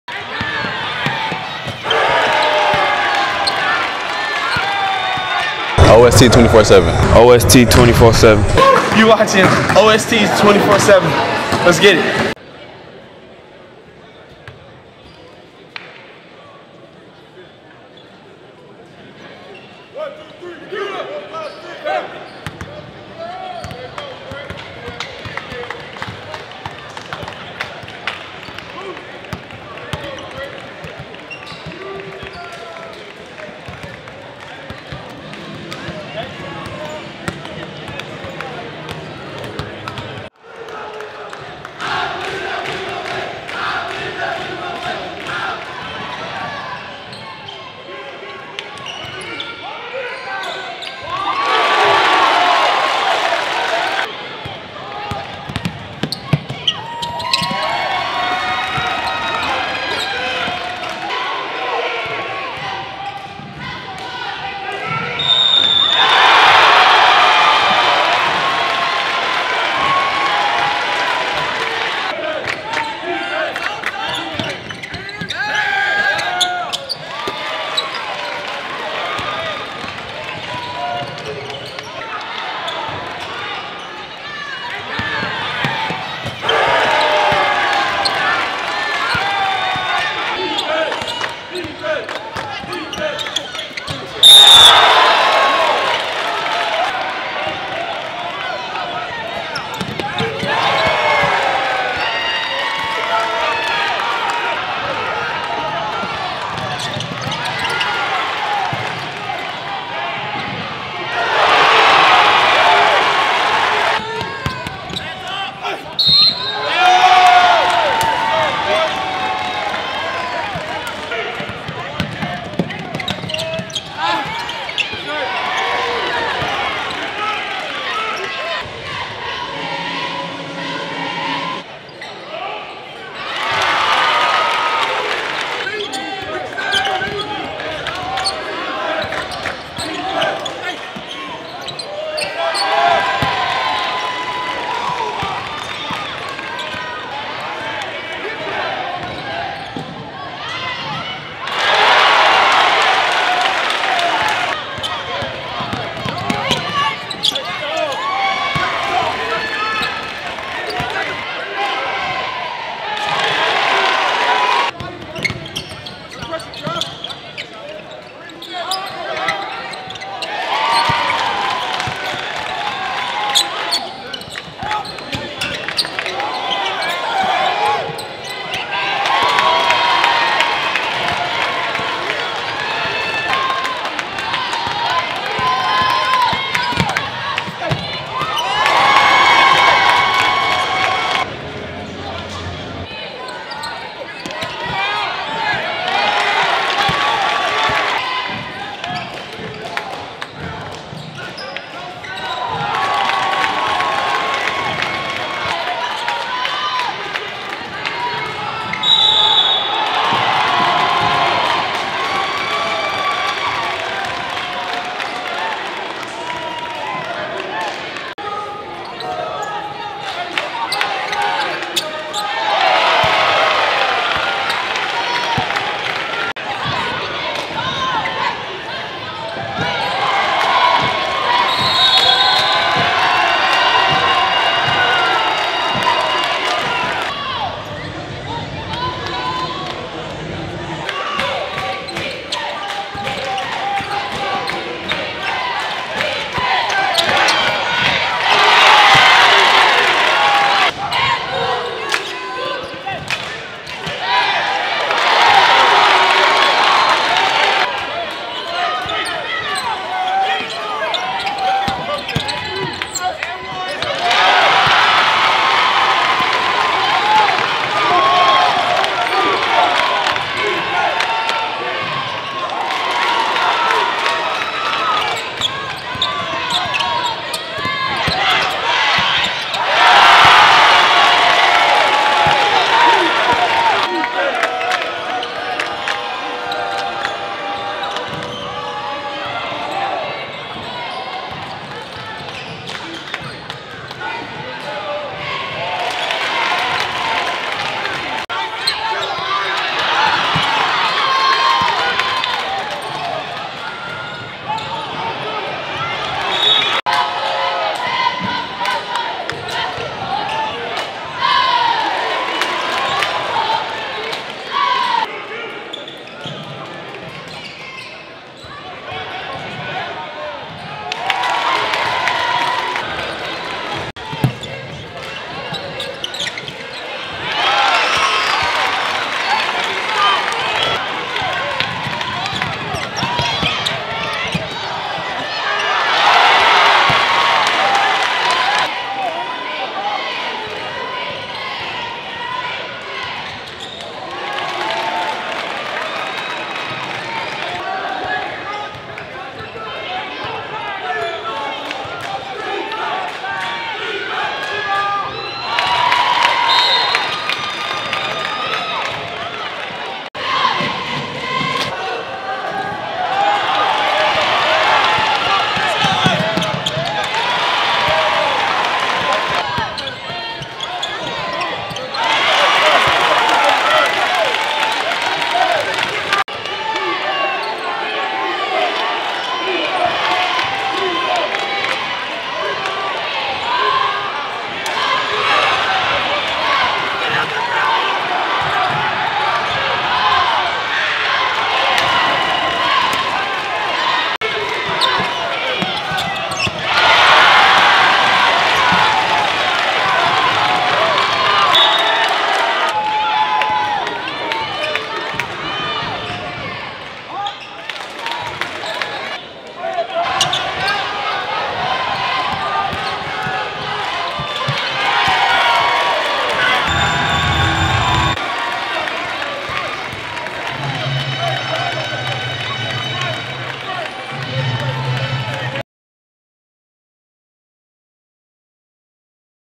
ALEXANDER & DACULA BATTLE FOR A SWEET 16 STATE PLAYOFF SPOT
For Alexander, CJ Hawkins 19pts  Malik Battle 12pts Bryce Robinson 11pts

For Dacula, Mekhail Bethea 19pts Brenden Tucker 16pts Quincy Ademokoya 12pts

AFTER YOU FINISH, DON’T FORGET TO HIT THAT THUMBS UP BUTTON UP BUTTON

THANKS FOR ROCKING WITH US